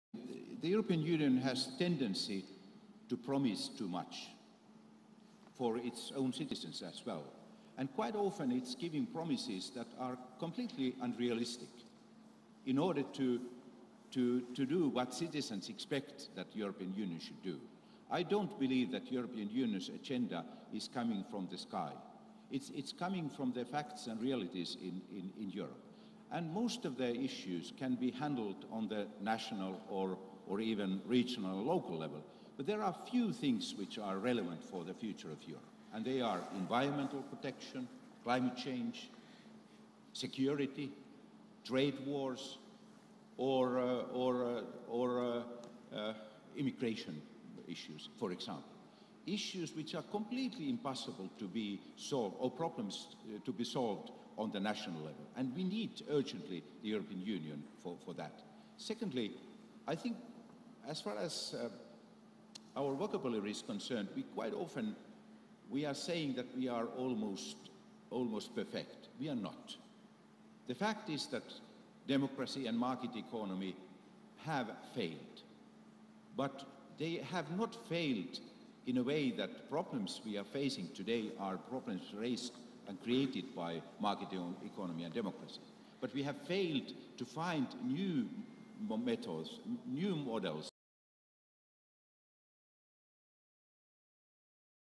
Highlights: "The EU often makes promises it can't keep"Esko Aho - Former Prime Minister of Finland
“The EU has the tendency to promise too much - for its own citizens, as well… We are saying we are almost ‘perfect’. We are not. The fact is, democracy and market economy have failed.” - Esko Aho - Former Prime Minister of Finland at TRT World Forum 2019